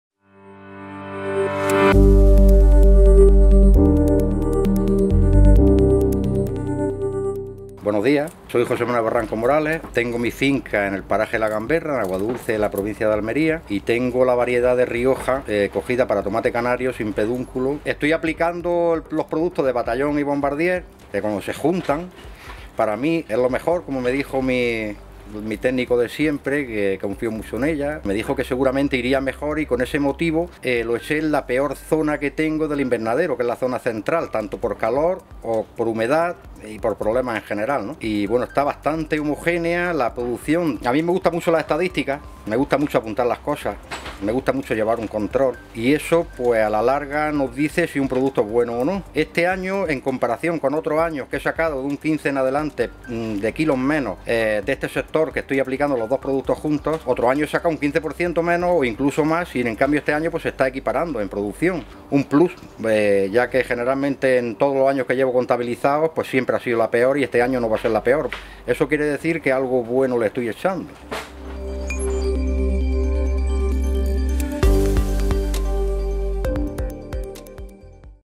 Testimonio Agricultor Tomate - España
El agricultor tras la aplicación de Batallón y Bombardier en la zona con mayor problemática de la finca, ha conseguido equiparar la producción al resto de la finca que estaba en mejor condiciones y no han sido aplicados ambos productos.
Batallón es un bioestimulante de suelos que mejora la producción y precocidad de las cosechas.
Bombardier es un bioestimulante de acción radicular y foliar que aporta energía, vigor, rápida respuesta frente a estrés y crecimiento en la planta.